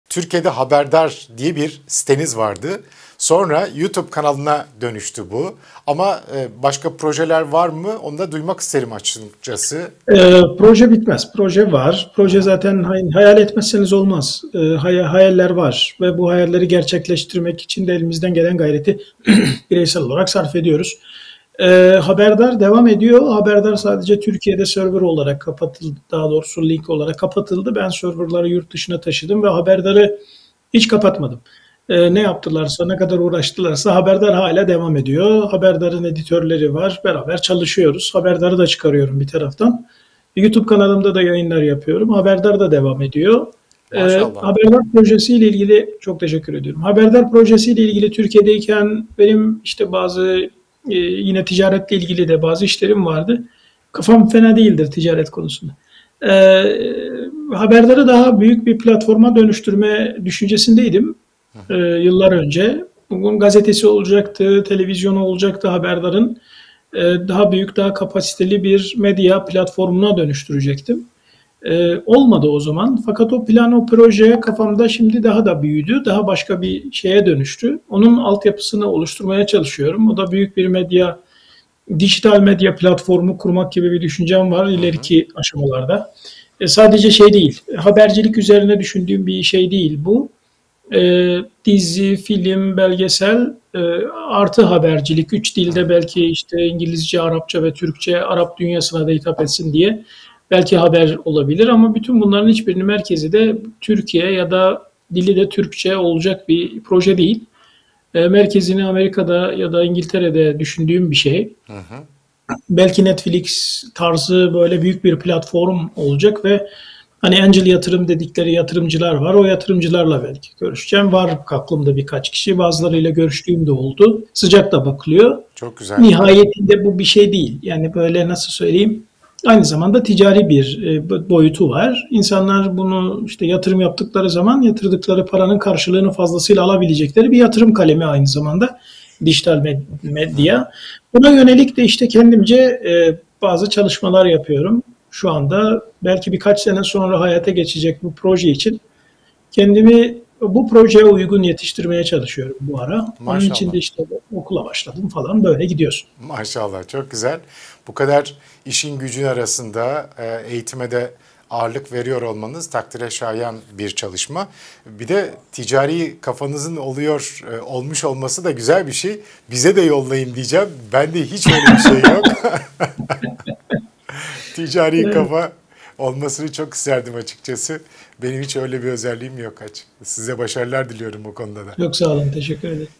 SAİD SEFA'NIN HEDEFİ NE? | KONUŞMA ZAMANI Özel
Gazeteci Said Sefa ile geçen hafta yaptığımız yayından kısa bir bölüm.
Said Sefa Bey, önümüzdeki yıllarda gerçekleştirmeyi planladığı hedeflerini anlattı.

Önemli not:
YouTube'da başka kanalımız yoktur :)

*** ÇOK TEŞEKKÜR EDİYORUM ***